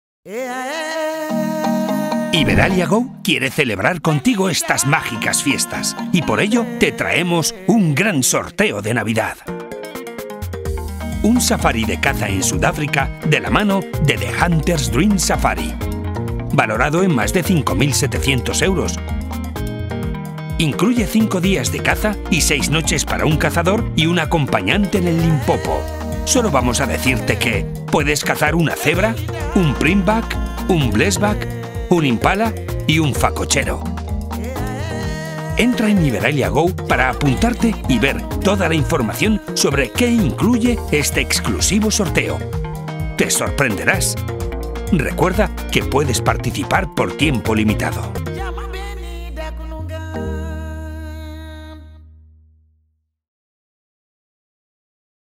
Un año más THE HUNTER’S DREAM SAFARI e IBERALIA GO sortean El SAFARI de CAZA en SUDÁFRICA que sueñas
Amigos,
Un año más THE HUNTER’S DREAM SAFARI tiene el placer de sortear junto IBERALIA GO:
El SAFARI de CAZA en SUDÁFRICA con el que llevas soñando hace tanto tiempo.
El afortunado cazará junto a ANTONIO MORA, uno de los pocos cazadores profesionales reconocidos en este mundo de la caza.
Sus consejos y su pericia en el campo harán que disfrutes como nunca y como nadie de estos días de lances y aventuras.
SIEMPRE HAY UN ANTES Y UN DESPUÉS cuando se caza con THE HUNTER’S DREAM SAFARI.

NO PUEDES PERDERTE ESTA OPORTUNIDAD. APÚNTATE en la aplicación de IBERALIA GO y ven a SUDÁFRICA con nosotros.